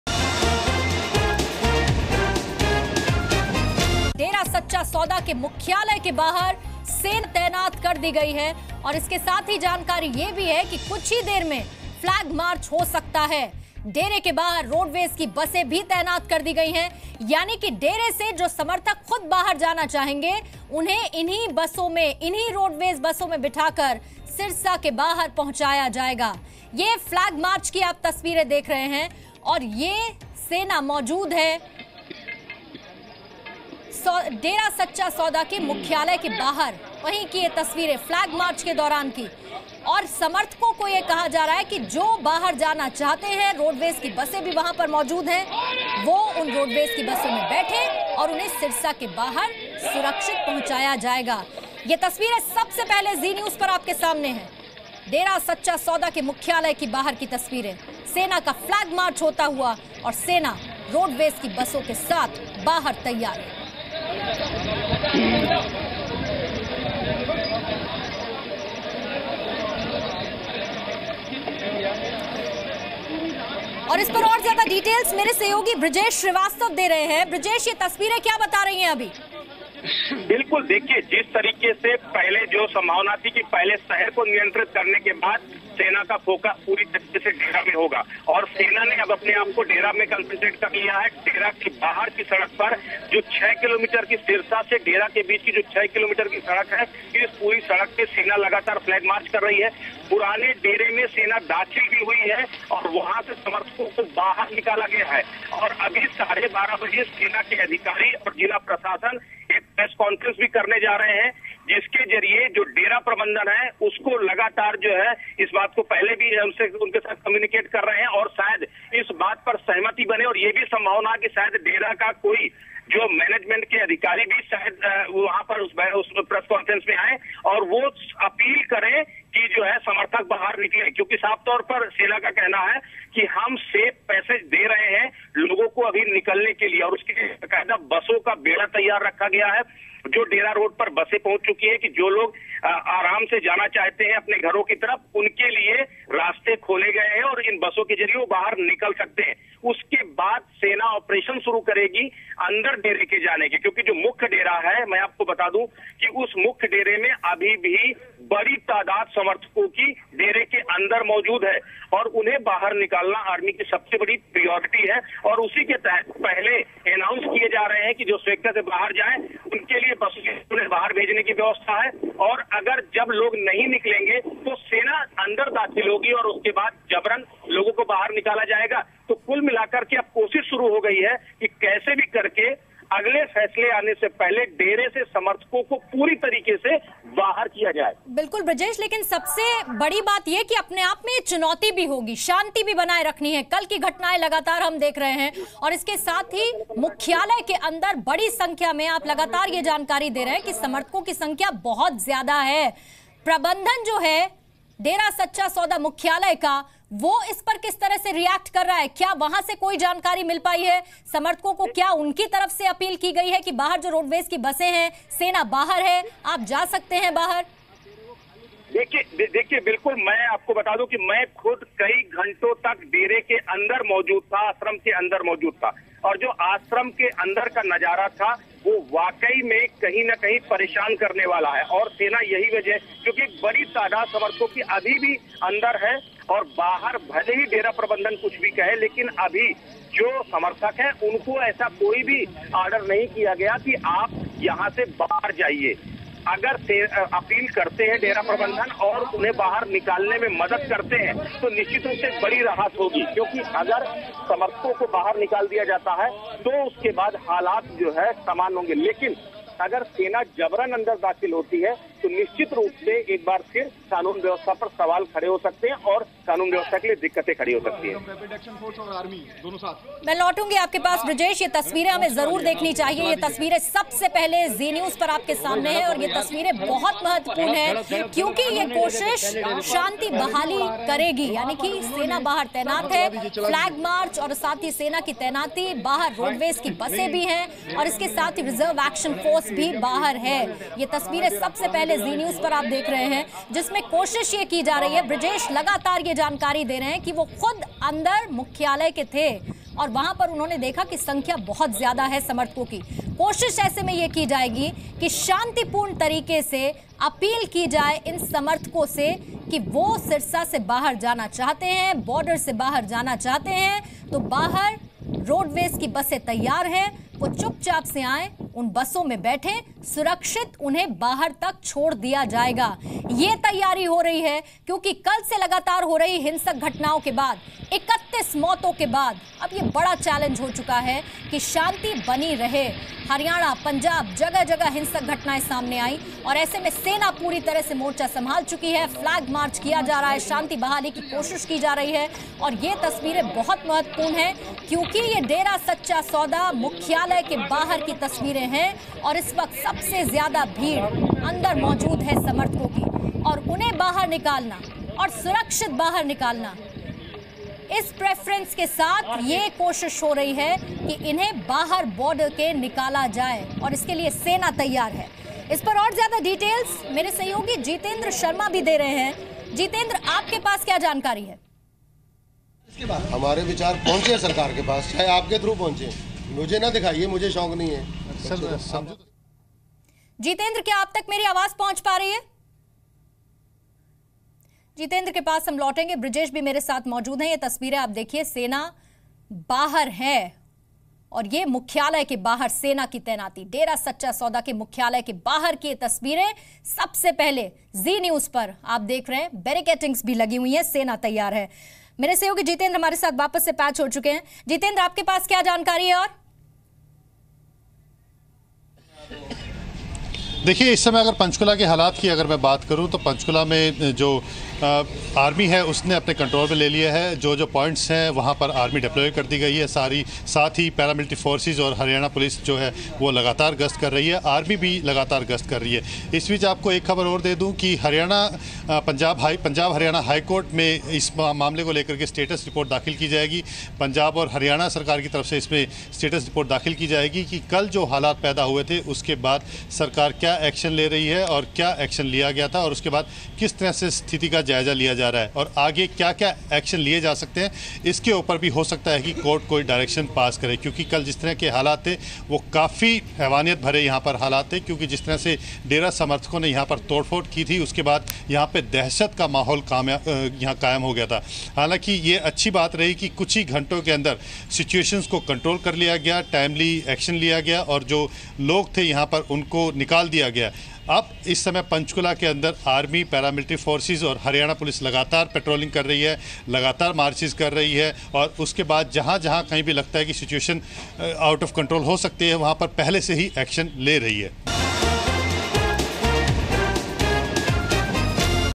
Watch: Army deployed outside Dera Sacha Sauda's headquarter| डेरा मुख्यालय के बाहर सेना तैनात
Army has been deployed outside Dera Sacha Sauda's headquarter in Sirsa. Army jawans conducted flag march and they will ask Dera followers to come out themselves. Buses have been arranged to take followers out of the city.

डेरा सच्चा सौदा के मुख्यालय के बाहर सेना तैनात कर दी गयी है। सेना डेरा में मौजूद लोगों से अपने आप बाहर आने के लिए कहेगी। डेरा के बाहर बसें मौजूद है जिनसे समर्थकों को सिरसा से बाहर ले जाया जाएगा।